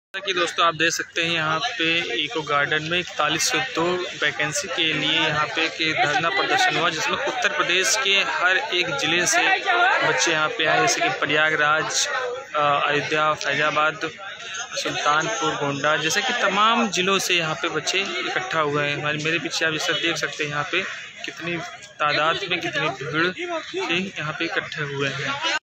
लखनऊ के इको गार्डन पार्क में UPPCL टेक्नीशियन #4102 वैकेंसी की बहाली के लिए धरना प्रदर्शन करते हुए
यूपीपीसीएल टेक्निशियन 4102 सीट के लिए धरना प्रदर्शन लखनऊ के इको गार्डन
UPPCL TG2 4102 महा आंदोलन  UPPCL TG2 4102 महा आंदोलन | ECO Garden Lucknow Se Live  | uppcl tg2 result 2021 जारी करो | 9 august